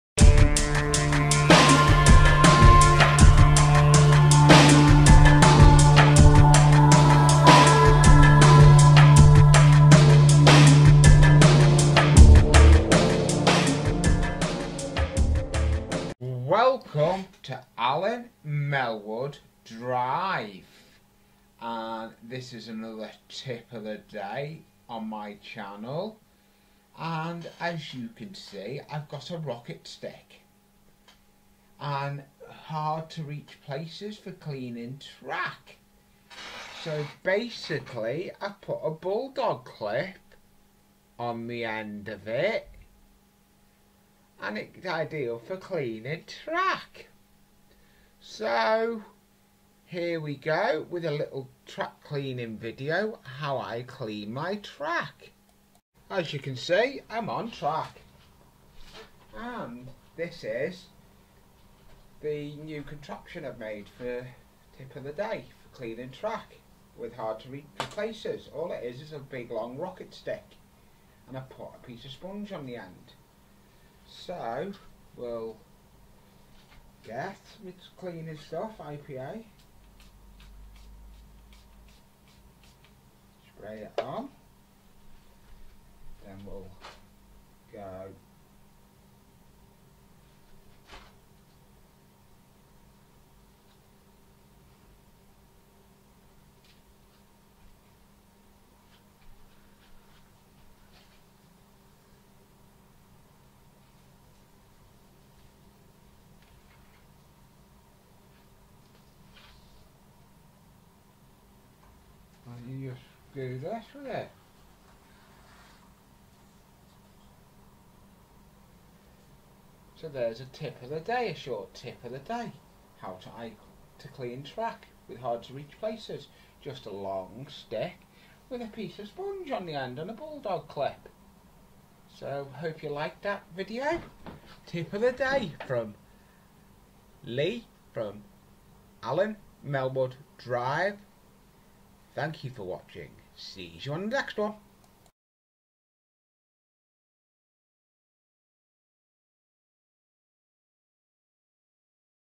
alan melwood drive layout tip of day
tip of day how clean track in  hare place you can not get to with your hand